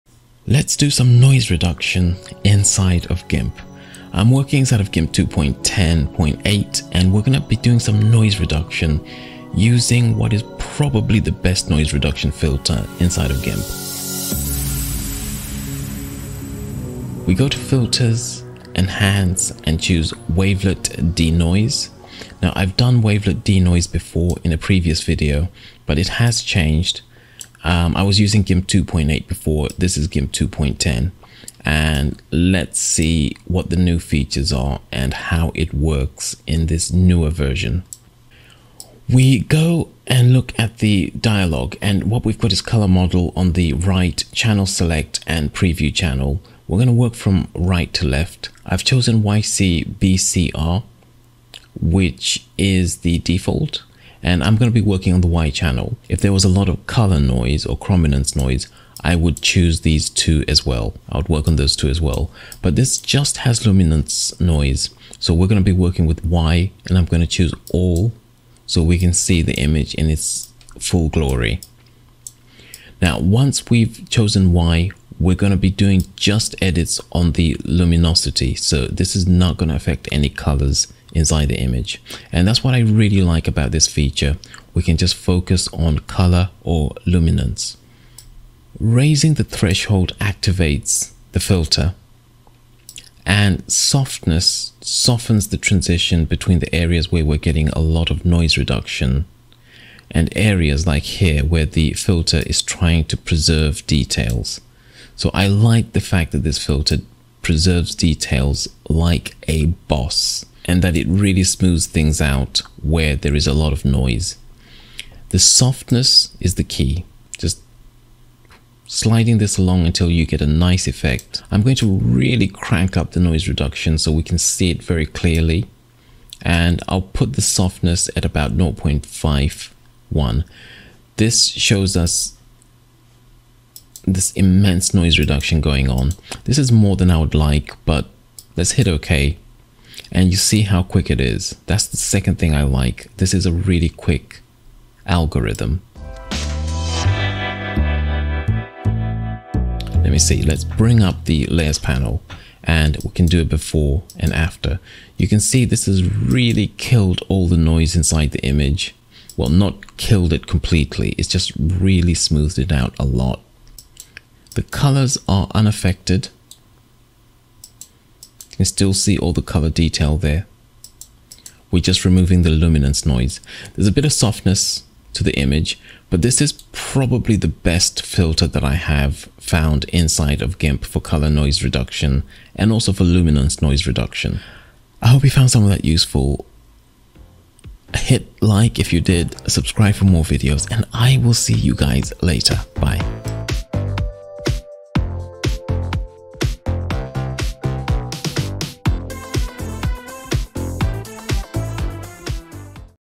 Remove Noise with Wavelet Denoise in Gimp 2.10
It has been a half decade since I last looked at Wavelet Denoise in Gimp 2.8.

The plugin has been updated since and in this video we take a look to see how the newer version performs in 2019.

Bear in mind that trying to remove noise inside a video never does any filter true justice.  You really need hands-on experience to truly get a sense of the accomplishment.

"Save it to the plugins folder and it should just work. If you need help theres a very good unofficial gimp page on facebook. The community are welcoming and helpful"